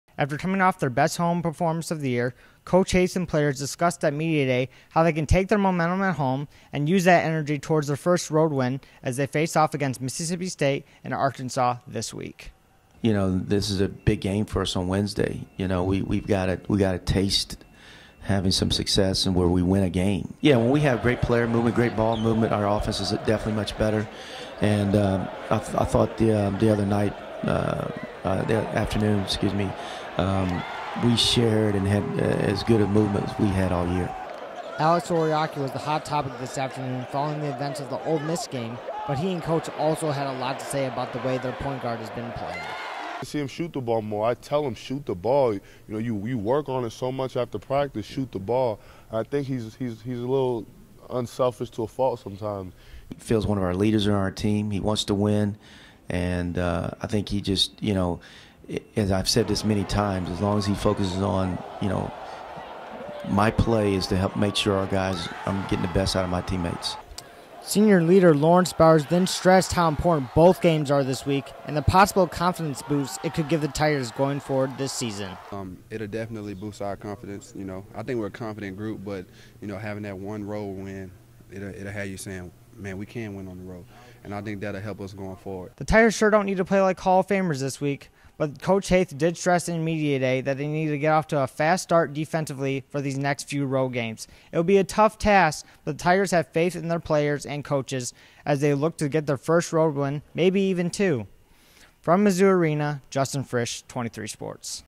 Basketball Media Day 2-11-13
Justin Frisch brings you the best sound bites and updates from Missouri men's basketball's media day after their home win over Ole Miss.